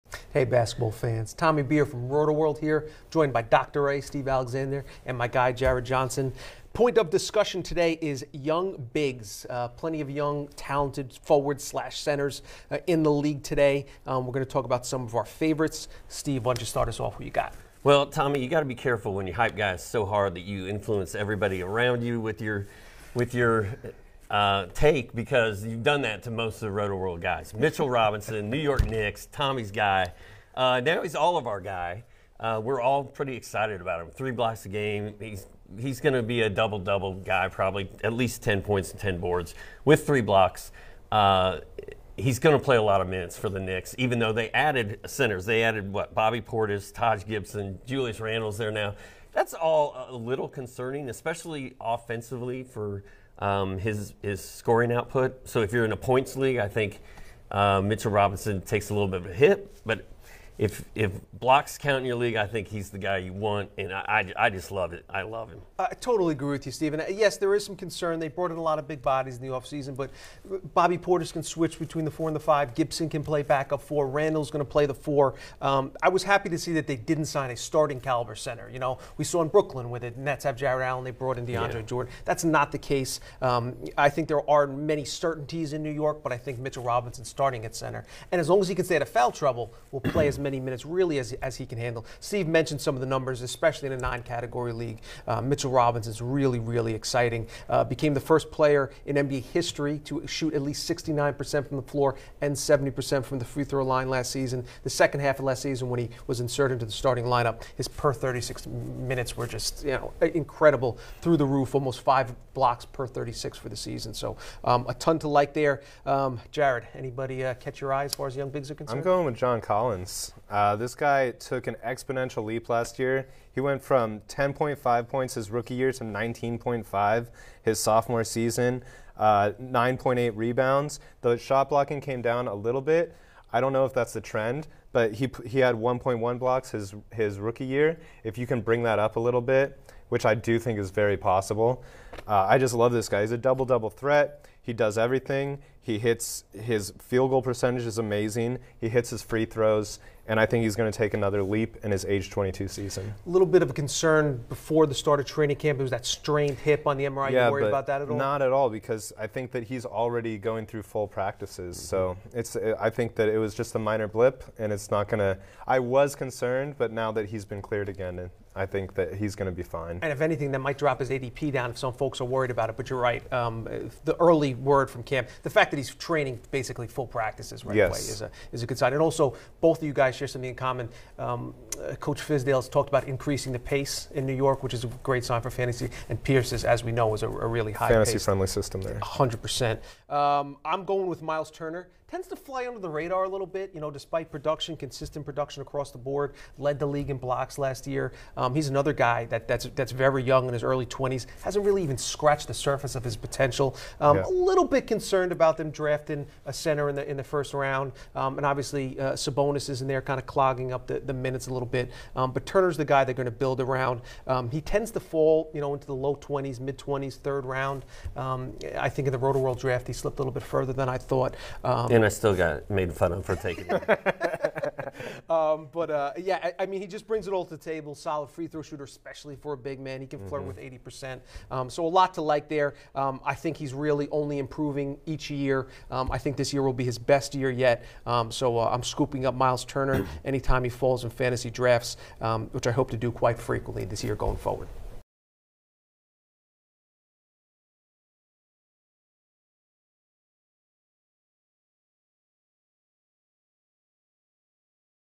Who are the best of the young, big men in the NBA? | ROTOWORLD
Mitchell Robinson brings a lot of excitement to New York and John Collins is rebounding from an injury nicely in Atlanta, but there are several other fantasy big men to watch in the upcoming season.